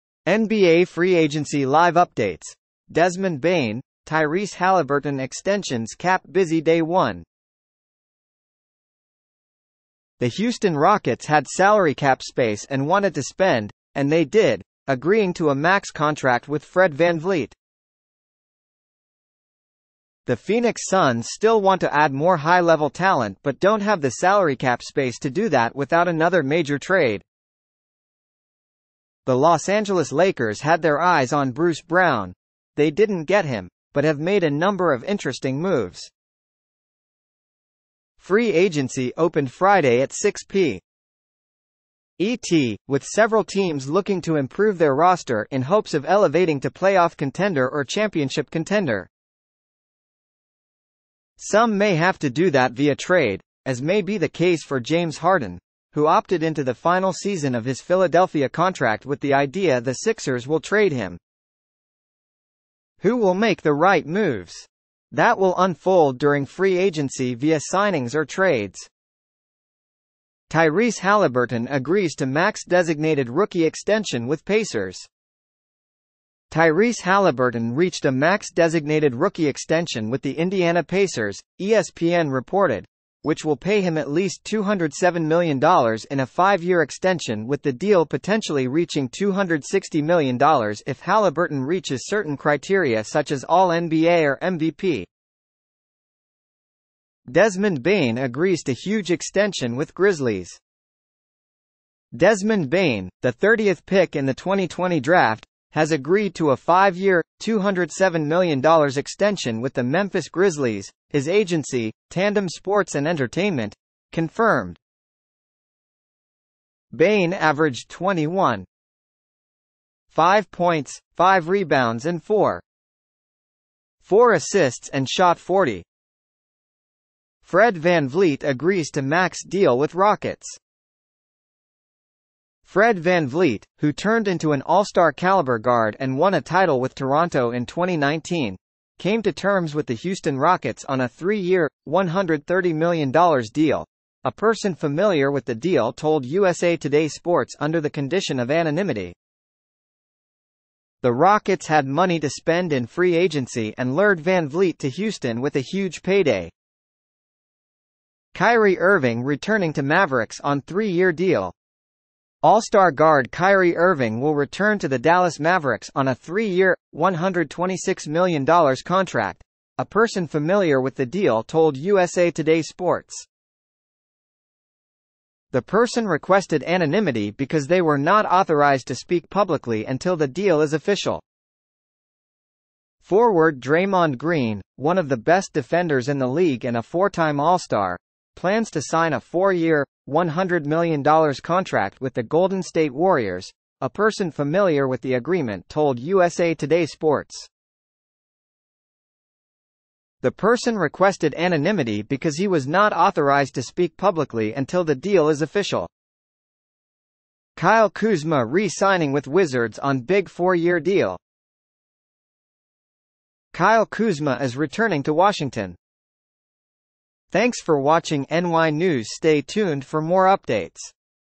NBA free agency live updates Desmond Bane, Tyrese Haliburton extensions cap busy Day 1 | NY News
The Houston Rockets had salary cap space and wanted to spend — and they did, agreeing to a max contract with Fred VanVleet. The Phoenix Suns still want to add more high-level talent but don’t have the salary cap space to do that without another major trade. The Los Angeles Lakers had their eyes on Bruce Brown; they didn't get him, but have made a number of interesting moves.
Free agency opened Friday at 6 p.m. ET, with several teams looking to improve their roster in hopes of elevating to playoff contender or championship contender. Some may have to do that via trade, as may be the case for James Harden, who opted into the final season of his Philadelphia contract with the idea the Sixers will trade him.
Who will make the right moves? That will unfold during free agency via signings or trades.
Tyrese Haliburton agrees to max designated rookie extension with Pacers
Tyrese Haliburton reached a max designated rookie extension with the Indiana Pacers, ESPN reported, which will pay him at least $207 million in a five-year extension with the deal potentially reaching $260 million if Haliburton reaches certain criteria such as All-NBA or MVP.
Desmond Bane agrees to huge extension with Grizzlies
Desmond Bane, the 30th pick in the 2020 draft, has agreed to a five-year, $207 million extension with the Memphis Grizzlies, his agency, Tandem Sports and Entertainment, confirmed. Bane averaged 21.5 points, 5 rebounds and 4.4 assists and shot 40.8% from 3 last season.
Fred VanVleet agrees to max deal with Rockets
Fred VanVleet, who turned into an All-Star caliber guard and won a title with Toronto in 2019, came to terms with the Houston Rockets on a three-year, $130 million deal, a person familiar with the deal told USA TODAY Sports under the condition of anonymity. The Rockets had money to spend in free agency and lured VanVleet to Houston with a huge payday.
Kyrie Irving returning to Mavericks on three-year deal
All-Star guard Kyrie Irving will return to the Dallas Mavericks on a three-year, $126 million contract, a person familiar with the deal told USA TODAY Sports. The person requested anonymity because they were not authorized to speak publicly until the deal is official.
Draymond Green agrees to new deal with Warriors
Forward Draymond Green, one of the best defenders in the league and a four-time All-Star, plans to sign a four-year, $100 million contract with the Golden State Warriors, a person familiar with the agreement told USA TODAY Sports. The person requested anonymity because he was not authorized to speak publicly until the deal is official.
Khris Middleton, Bucks agree to new deal
Khris Middleton will return to the Milwaukee Bucks on a three-year, $102 million contract, his agent, Jeff Schwartz, confirmed to USA TODAY Sports.
Kyle Kuzma re-signing with Wizards on big four-year deal
Kyle Kuzma is returning to Washington. Kuzma agreed to a four-year, $102 million deal Friday to remain with the Wizards, a person familiar with the agreement told USA TODAY Sports. Kuzma averaged 21.2 points, 7.2 rebounds and 3.7 assists last season on the Wizards, a team that appeared to be in rebuild mode after offloading several players this offseason, including Bradley Beal. But Kuzma will not be one of them.
Wizards trade Monte Morris to Pistons
The Detroit Pistons acquired guard Monte Morris from the Washington Wizards for a second-round pick, a person with direct knowledge of the trade confirmed to USA TODAY Sports under the condition of anonymity. Last season, Morris averaged 10.3 points and 5.3 assists and shot 48% from the field and 38.2% on 3-pointers. The Wizards generated a $9.8 million trade exception they can use over the next year.
rizzlies agree to two-year deal with veteran Derrick Rose
Derrick Rose agreed to a two-year deal with the Memphis Grizzlies, according to ESPN. The 34-year-old veteran averaged 5.6 points, 1.5 rebounds and 1.7 assists in 27 games for the New York Knicks last season.
Raptors agree to two-year deal with Dennis Schroder
After Fred VanVleet agreed to a deal with the Houston Rockets, Dennis Schroder and the Toronto Raptors agreed to a two-year, $26 million contract, according to ESPN. Schroder, who averaged 12.6 points per game with the Los Angeles Lakers last season, helps fill the Raptors' need at point guard.
Kevin Love returning to Heat
Power forward Kevin Love is returning to the Miami Heat on a two-year deal, with the second year being a player option, The Associated Press reports. Love signed with the Heat after the Cleveland Cavaliers bought out his contract last season. The five-time All-Star started 18 games in the playoffs as the Heat advanced to the NBA Finals.
Seth Curry returning to Mavericks for third time
Guard Seth Curry has agreed to a two-year deal with the Dallas Mavericks, according to ESPN.